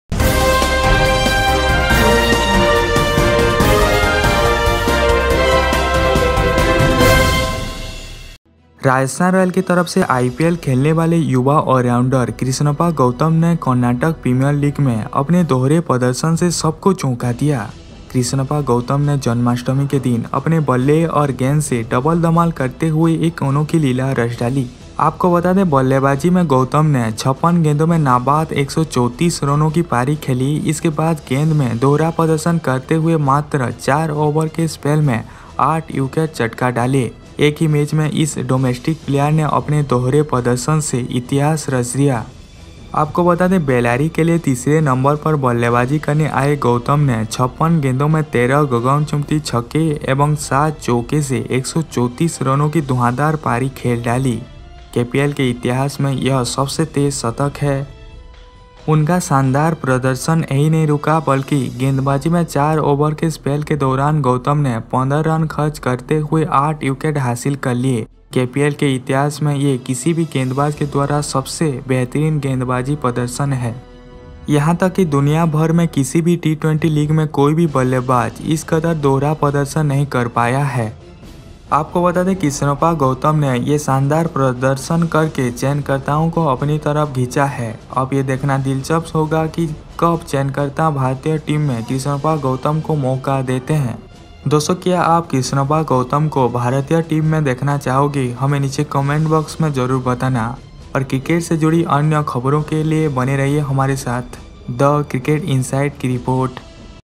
Krishnappa Gowtham Smashes Unbeaten Century, Takes Eight Wickets In Karnataka Premier League Match
Krishnappa Gowtham made history in the ongoing Karnataka Premier League (KPL), smashing the highest ever individual score in the tournament with a blistering 134 not out off just 56 balls that included seven fours and a whopping 13 sixes. Krishnappa Gowtham reached the magical three-figure mark in just 39 balls, the fastest hundred in the history of KPL. The Krishnappa Gowtham show didn't end there as the all-rounder followed it up with an incredible eight-wicket haul to take his team Bellary Tuskers to a 70-run win.

The 30-year-old's stunning exploits with both the bat and the ball helped his team Bellary Tuskers register a resounding win (VJD method) over the Shimoga Lions.